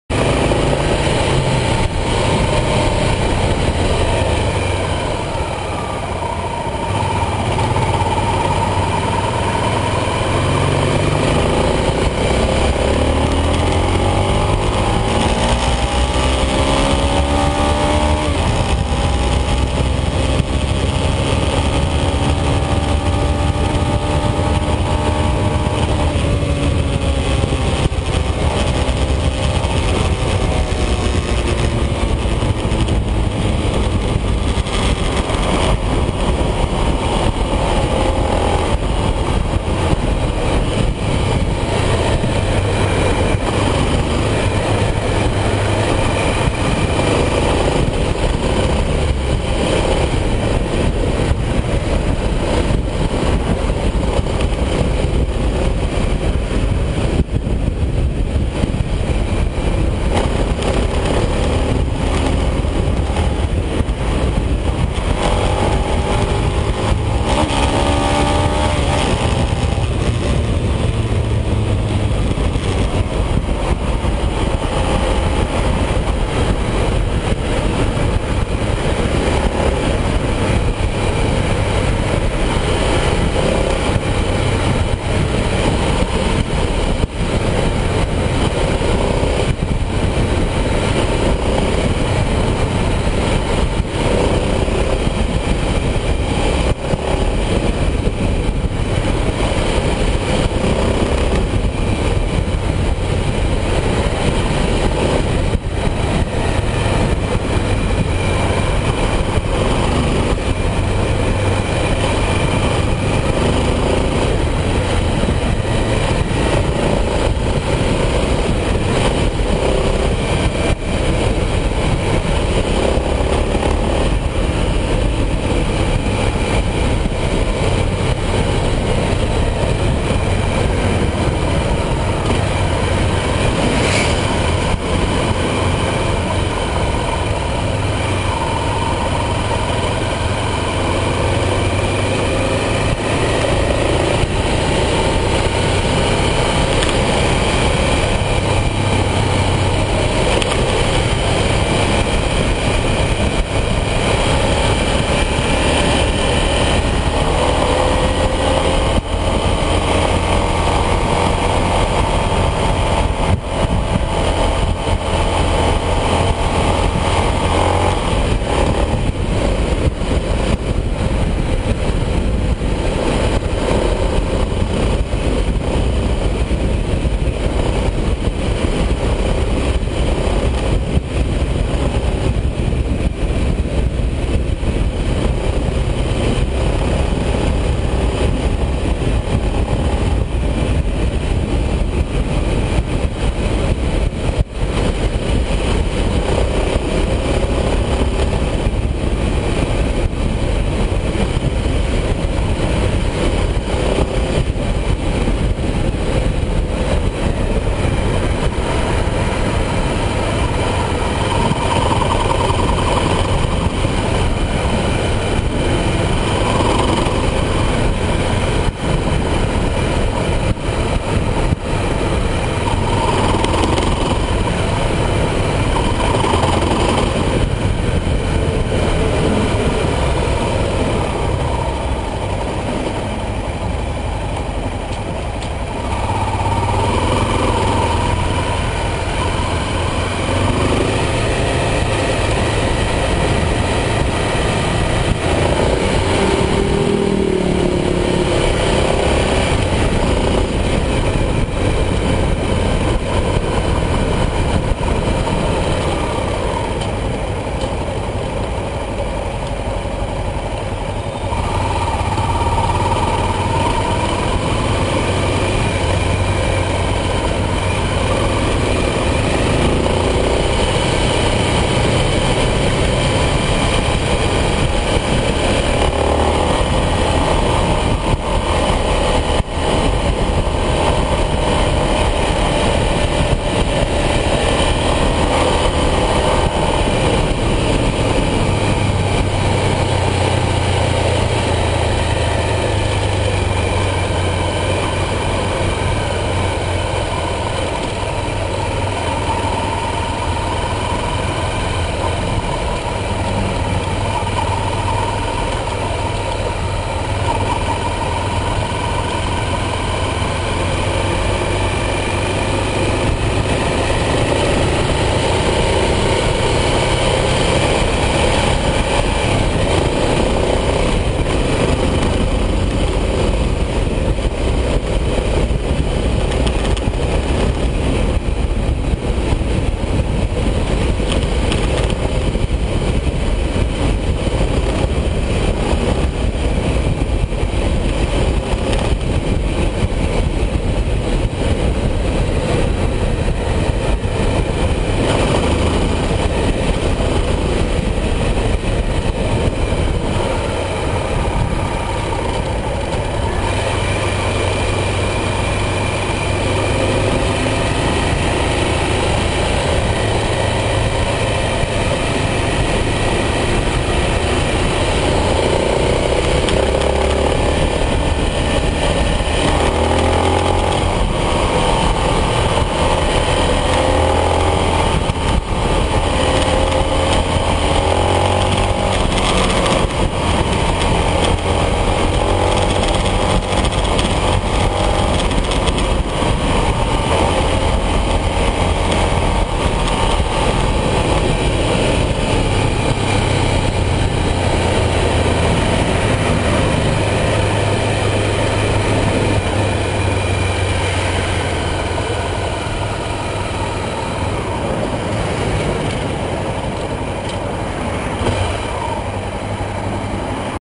XRE 300 - Role Part II
*** Assista, leia e Qualifique o video. ***
Mais um trecho do role.
Abraço

Moto: Honda XRE 300
         Honda CB 300

Câmera: GoPro Hero HD
Edição: Camtasia Studio 7